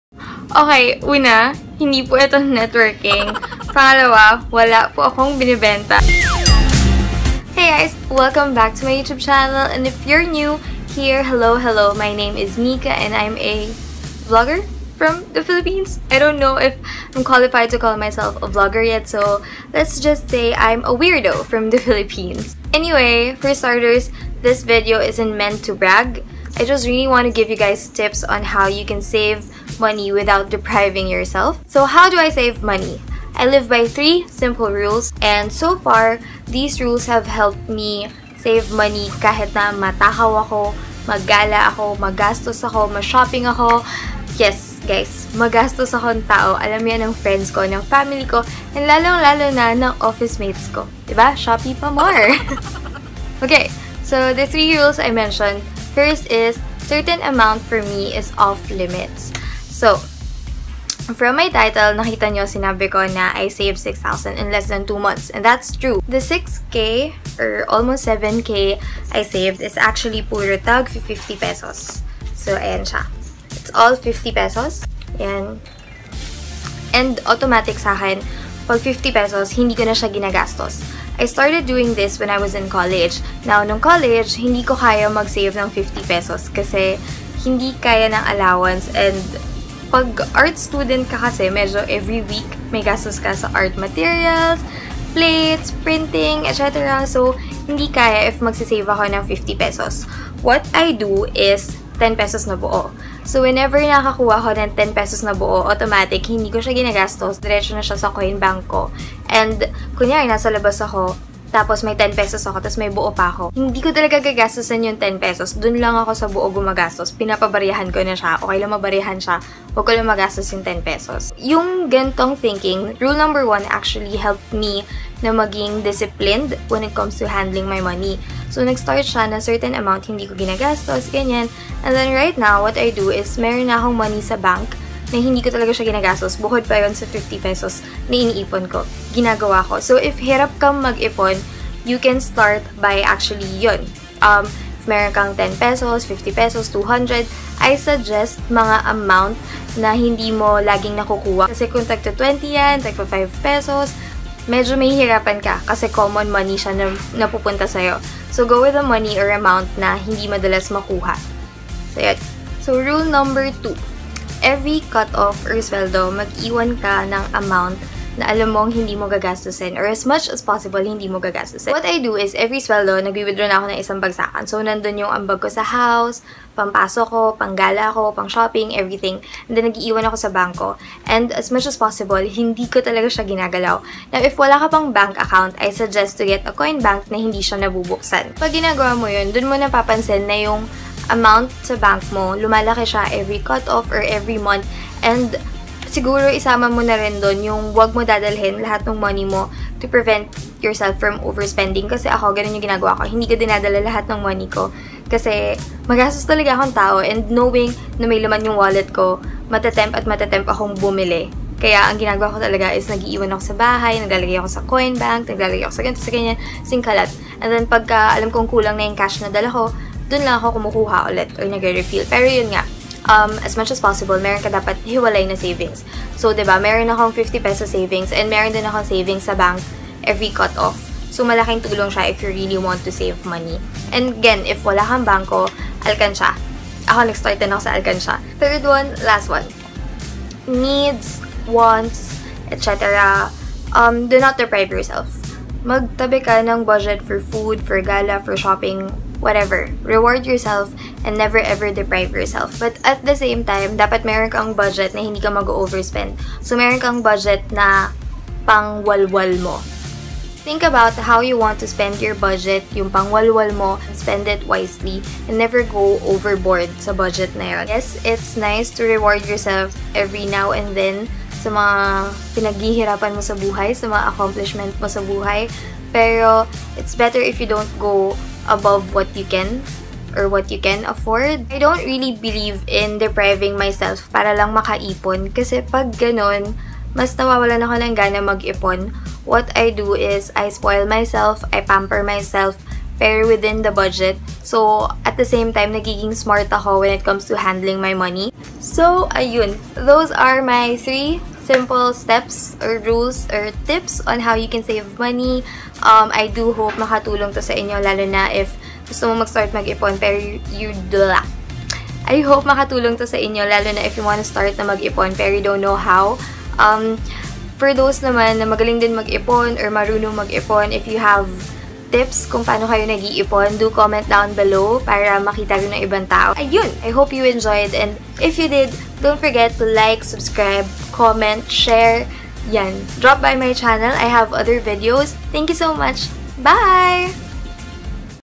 I saved P6k in less than 2 months! | ipon talk: paano nga ba mag ipon without depriving yourself?
Ipon talk! Hope this video helps ❤️❤️❤️